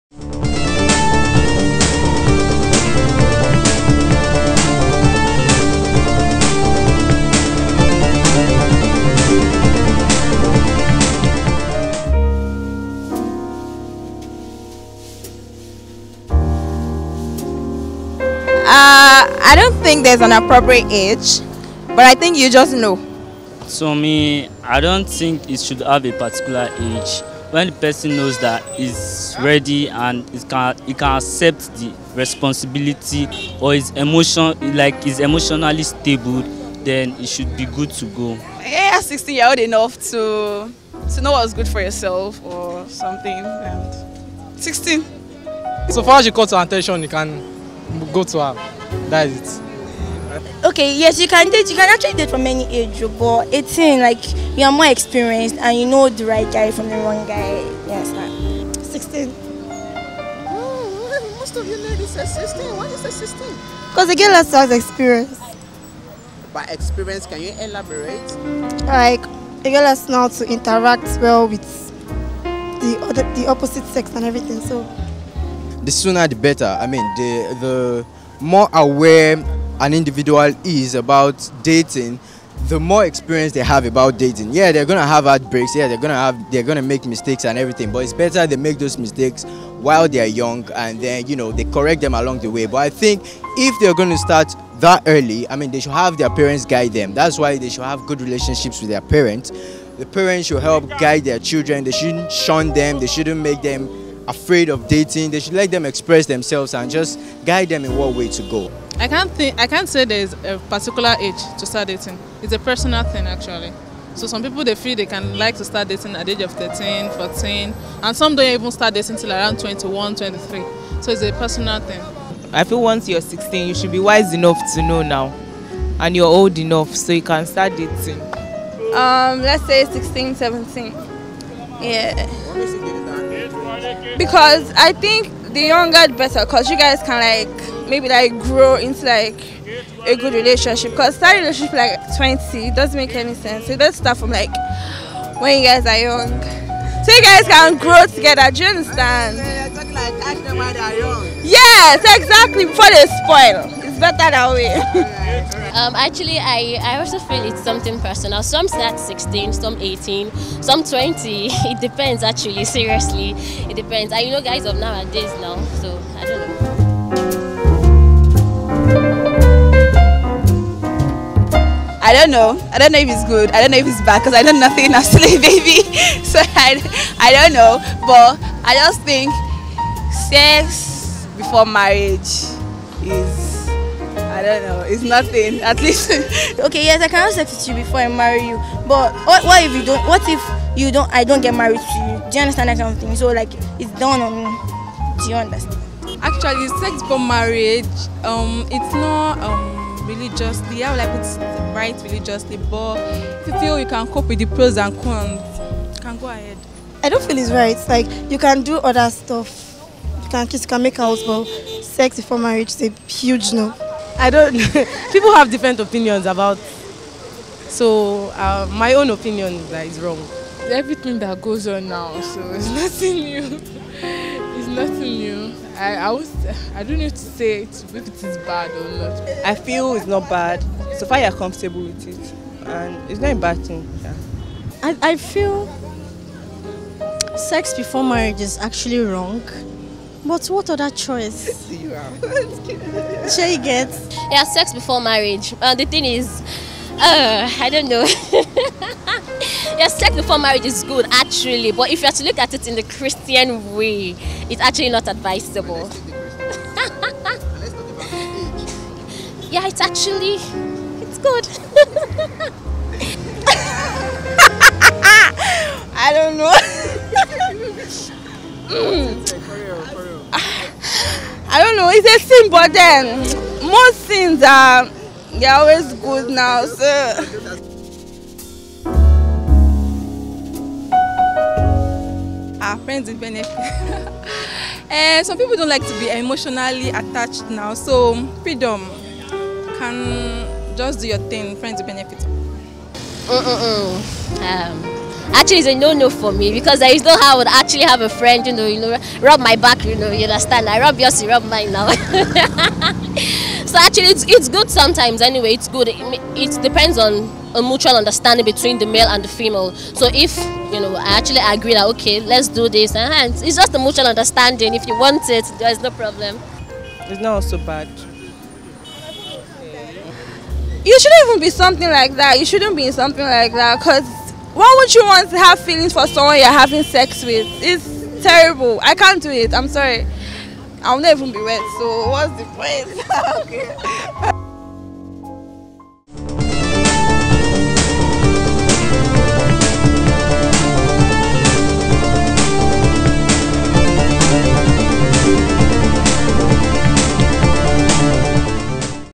RBPro Chat: The Right Age To Have A Boyfriend or Girlfriend I Nigeria Street interview I Funny video
In this funny episode of RBProChat, weekly Street Interview, Naija street Gist, Street Quiz of Ruggedboots Production, Nigeria, Africa, we ask; At what age is it appropriate to start dating.

Get ready for Funny responses!

The responses are differing, showing people understanding of this question, feel free to add your voice. The more, the merry.

Please Don't Forget to THUMBS UP, COMMENT & SHARE!